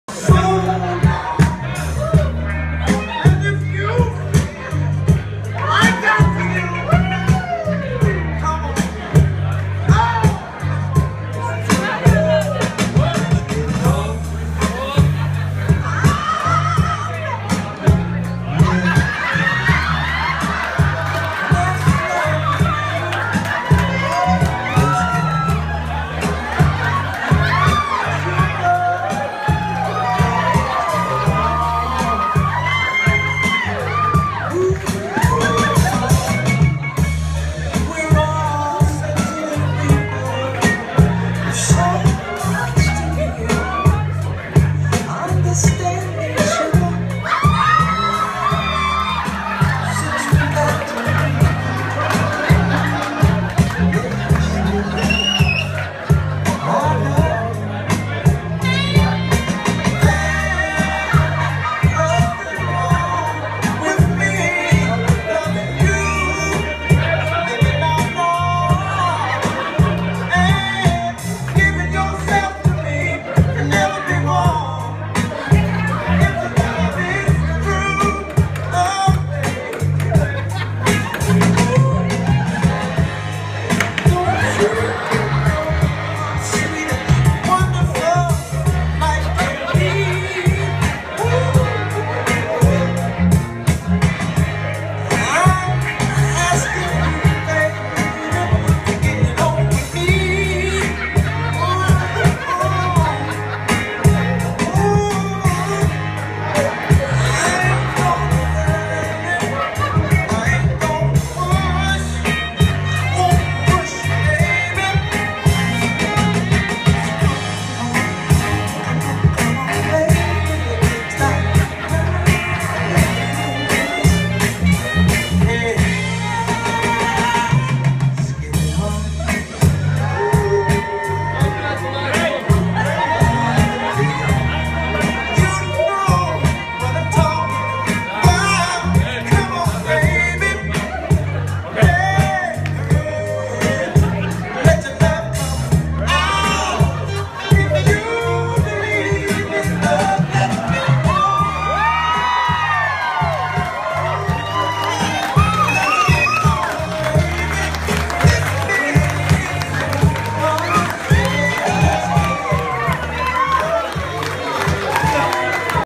Mechanic groom goes for the garter!
Taken at Tom & Kim's Wedding, August 15, 2015.